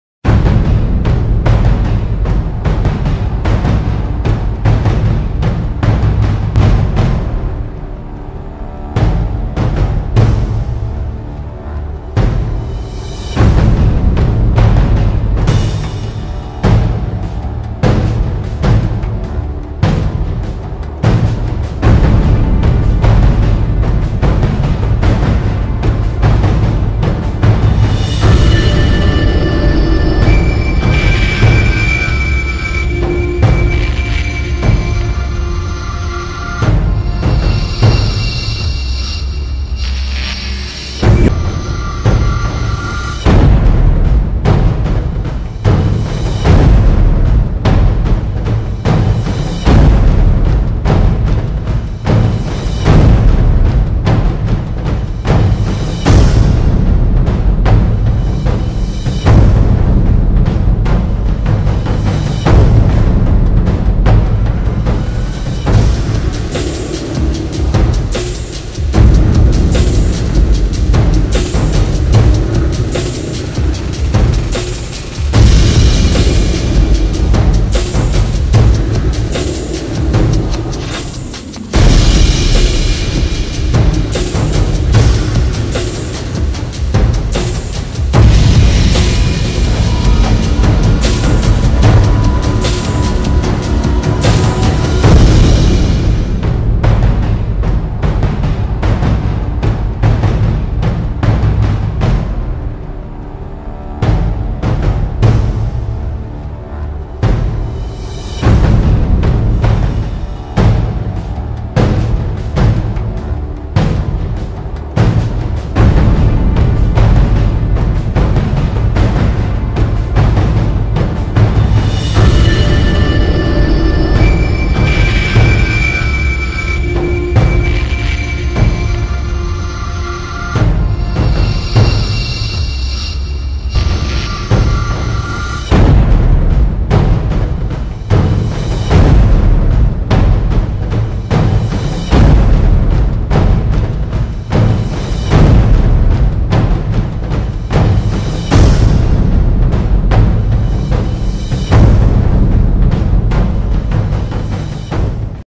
BATTLE OF IWOJIMA
A Man stands alone defending his self on the battlefield